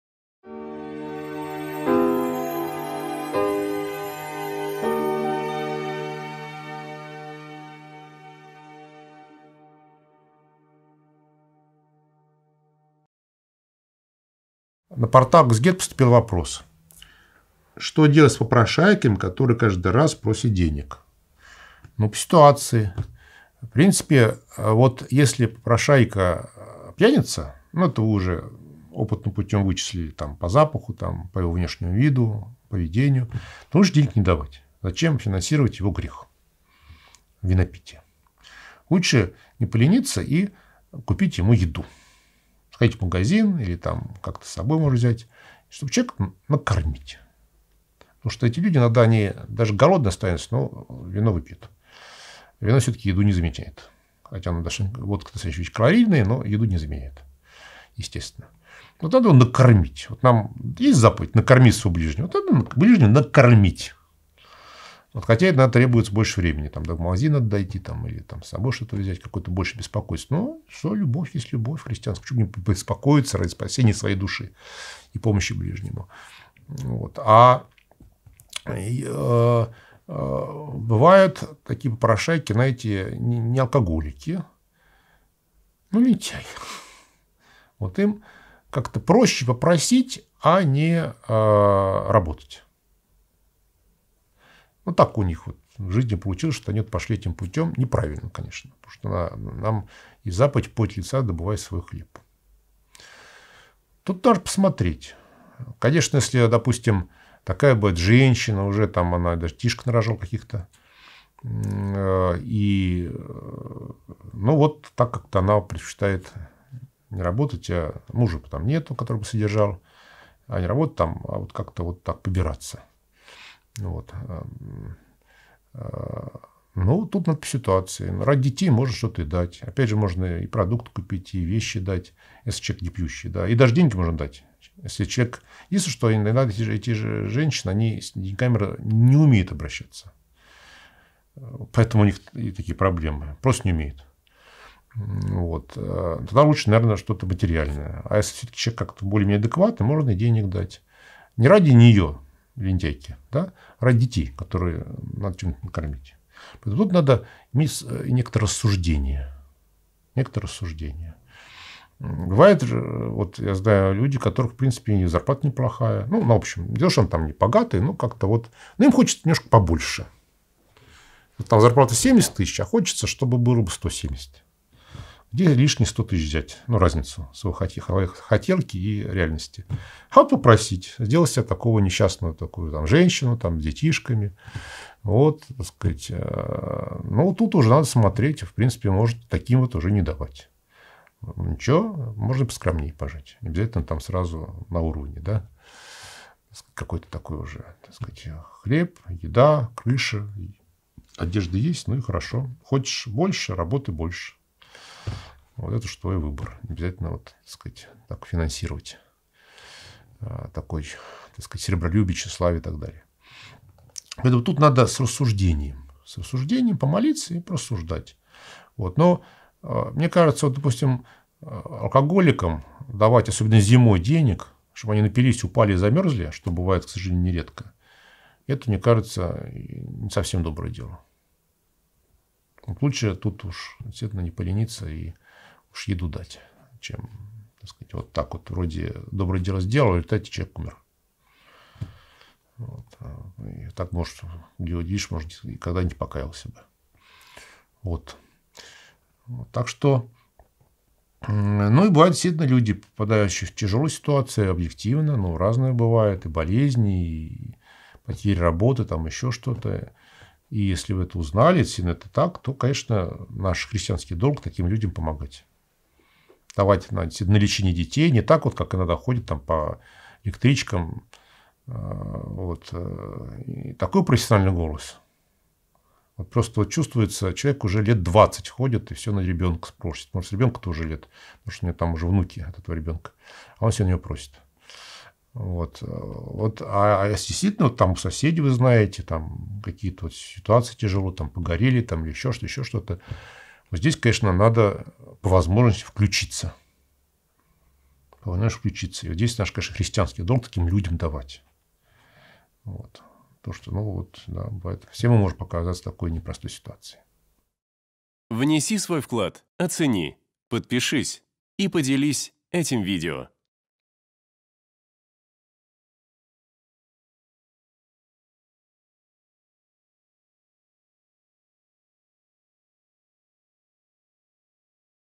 СТОИТ ЛИ ДАВАТЬ ДЕНЬГИ ПОПРОШАЙКАМ НА УЛИЦЕ ? Протоиерей Александр Никольский
Команда Экзегет.

В сегодняшнем видео на библейском портале Экзегет вместе с протоиереем Александром Никольским отвечаем на вопрос: "Стоит ли давать деньги попрошайкам на улице?"

🔸  Попрошайничество
🔸  Рассуждение
🔸  Милосердие